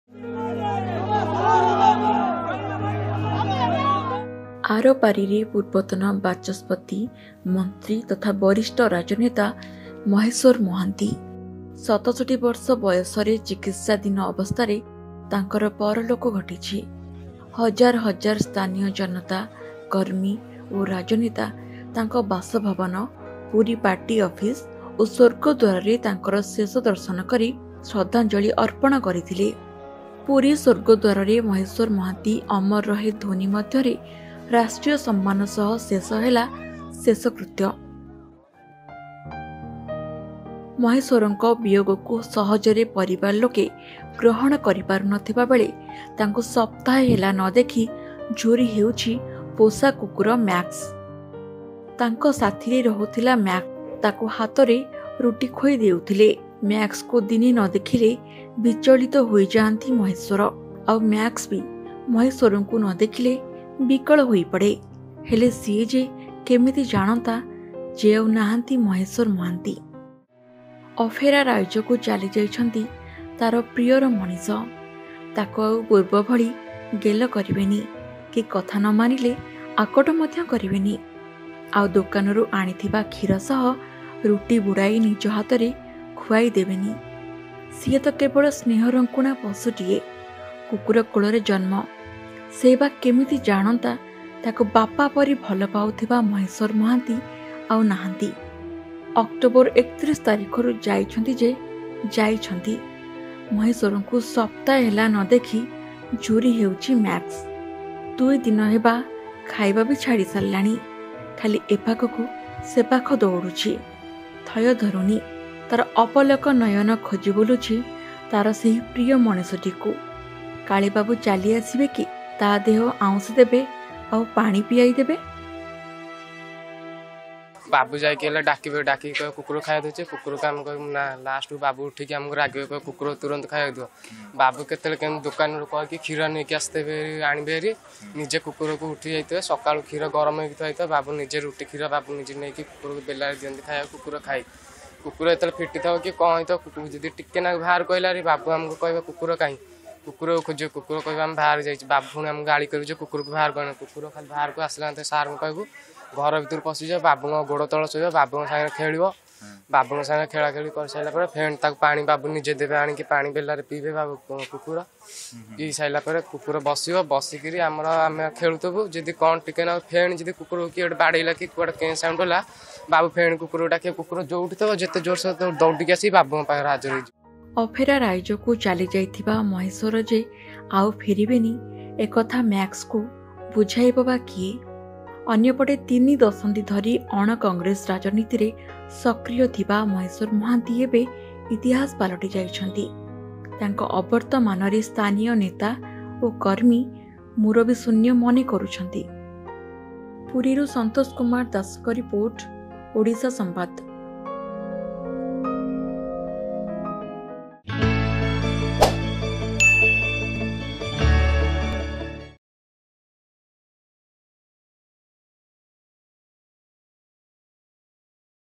Pet Dog Max is Remembering Maheswar Mohanty a Lot
ମହେଶ୍ବରଙ୍କ ବିୟୋଗକୁ ସହଜରେ ପରିବାର ଲୋକେ ଗ୍ରହଣ କରିପାରୁନଥିବାବେଳେ, ତାଙ୍କୁ ସପ୍ତାହେ ହେଲା ନଦେଖି ଝୁରି ହେଉଛି ପୋଷା କୁକୁର ମ୍ୟାକ୍ସ । ତାଙ୍କ ସାଥିରେ ରହୁଥିଲା ମାକ୍ସ । ତାକୁ ହାତରେ ରୁଟି ଖୋଇ ଦେଉଥିଲେ ।